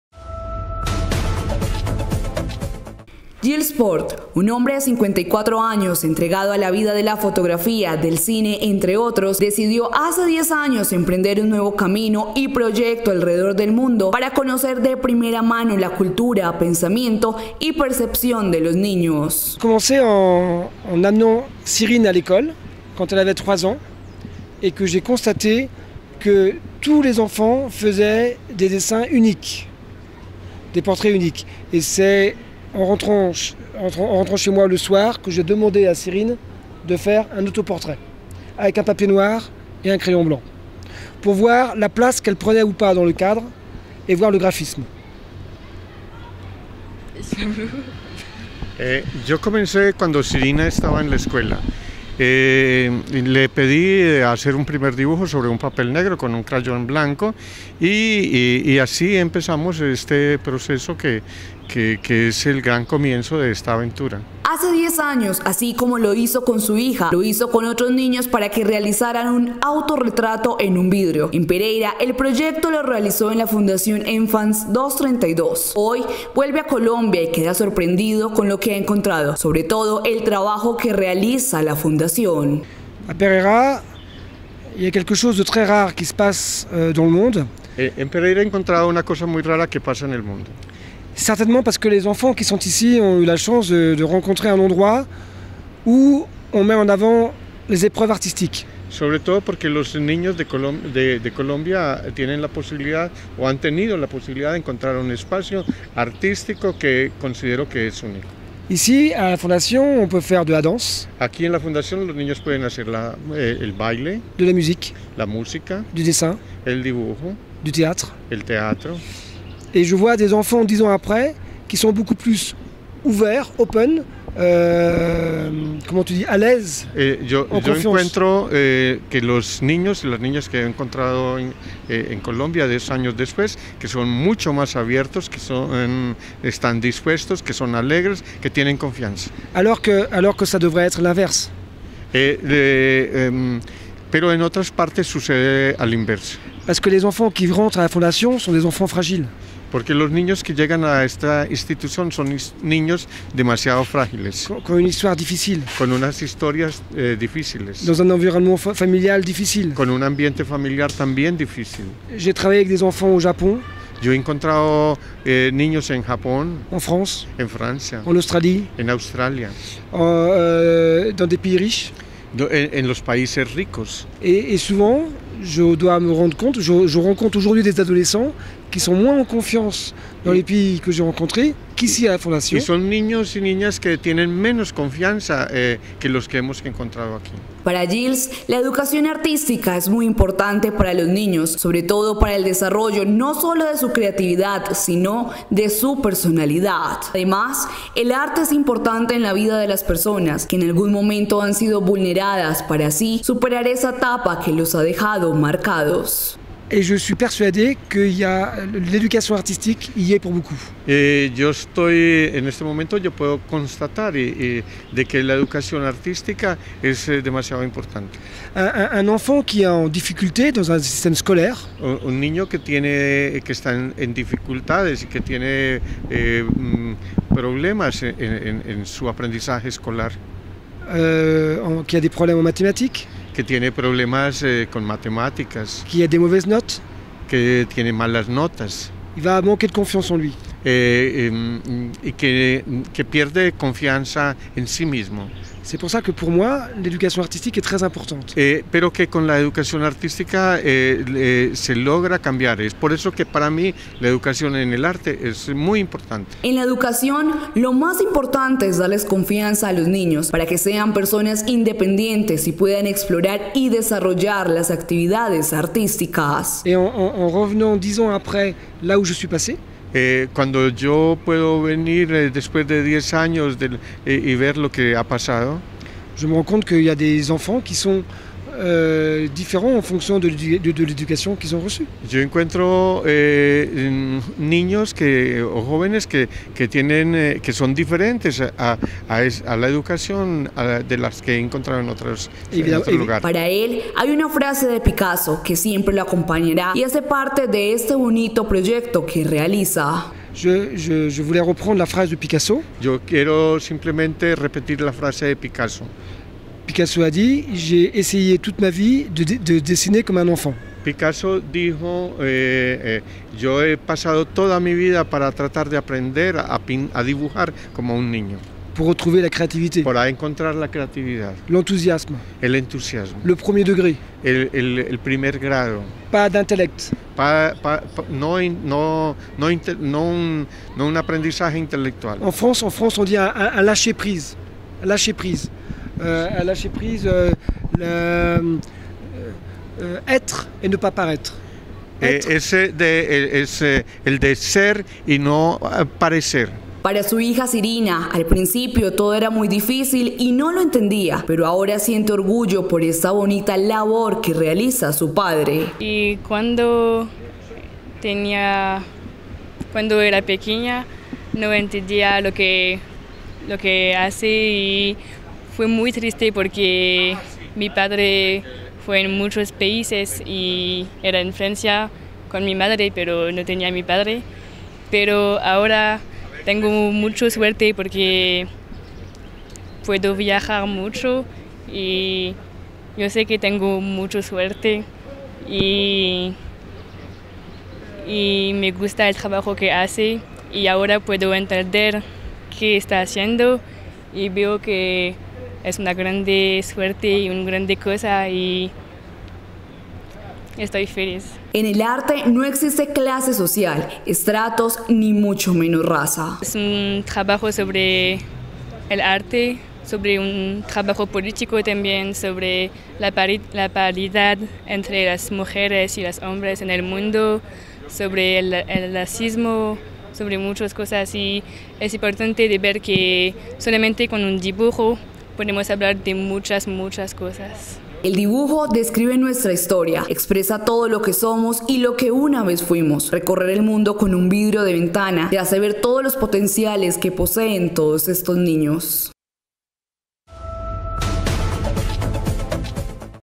La mirada de niños de todo el mundo reposa en la lente del director Gilles Porte
El director de cine y fotografía frances Gilles Porte trabaja desde hace 10 años en un ambicioso proyecto que recoge la mirada de niños de todo el mundo, entre ellos algunos pereiranos, Porte cuenta para CNCNoticias su experiencia en Colombia y la admiración que siente por el trabajo que realiza la Fundación Enfances232 que menores en condiciones vulnerables de la capital risaraldense.